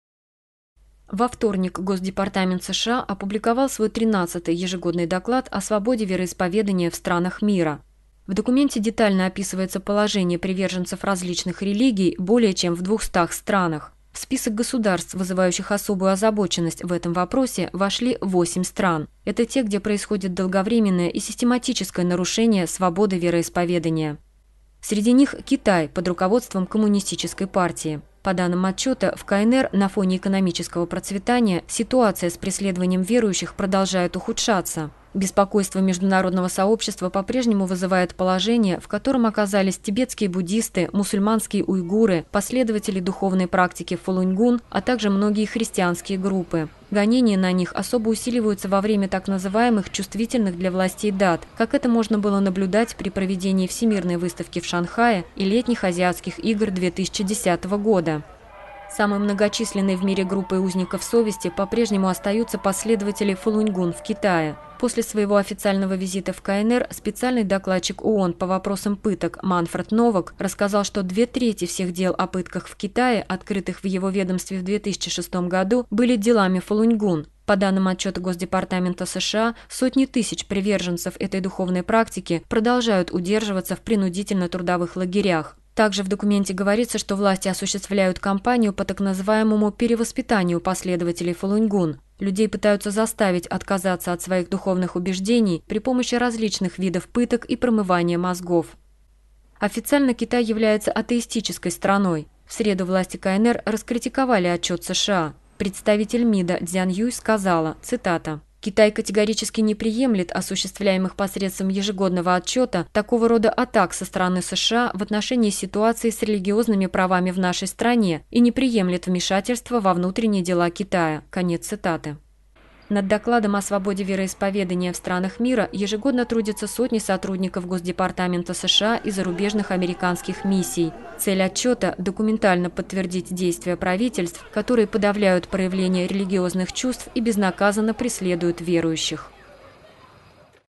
КНР опять в списке нарушителей свободы веры
) В опубликованном 13 сентября отчете Государственного департамента США детально описывается состояние своды вероисповедания в разных странах мира. В список стран, вызывающих особую озабоченность в этом вопросе, продолжает входить Китай.